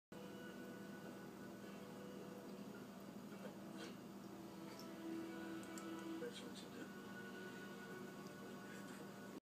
Dutchie Cat steals food
She's not supposed to eat this food, and she knows it!   Her collar would clink against the bowl so she learned to eat like a human!